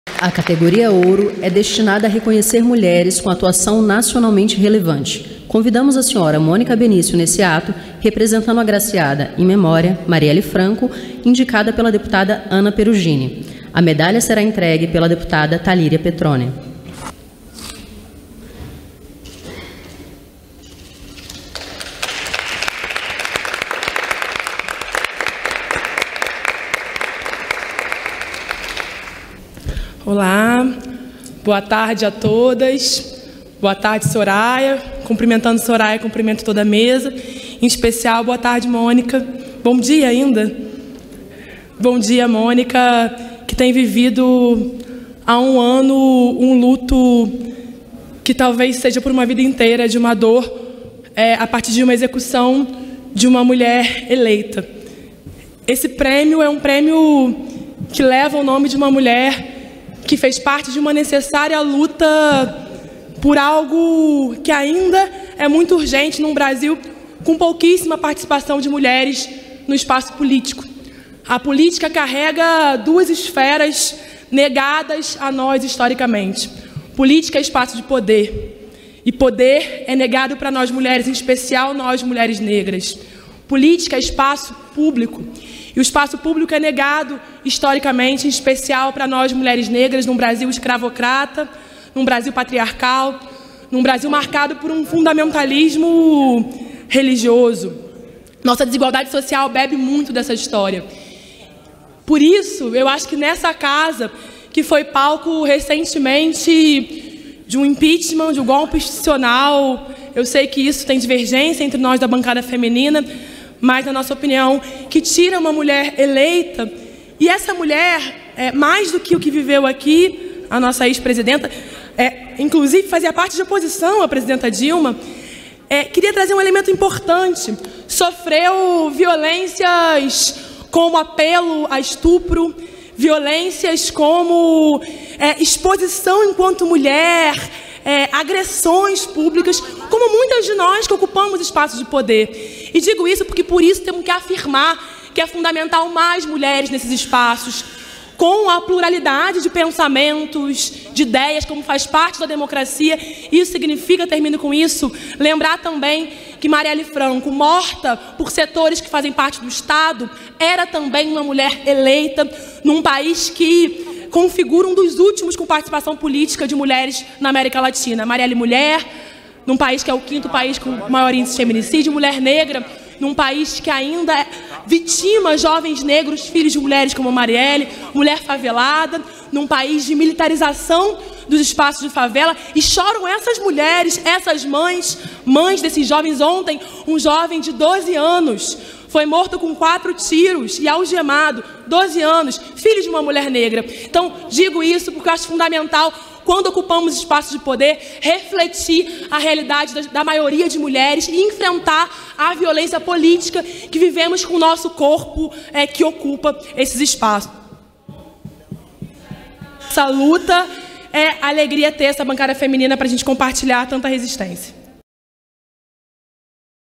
Marielle Franco é homenageada com Medalha Mietta Santiago
A Câmara dos Deputados, através da Secretaria da Mulher, agraciou Marielle Franco com a Medalha Mietta Santiago. A deputada do PSOL, Talíria Petrone (RJ), entregou a homenagem para Monica Benicio, viúva de Marielle. Durante a sessão, Talíria afirmou que as mulheres devem ocupar cada vez mais os espaços de poder. A condecoração foi criada no ano passado para homenagear iniciativas relacionadas aos direitos das mulheres e é entregue anualmente em março, em comemoração ao Dia Internacional da Mulher.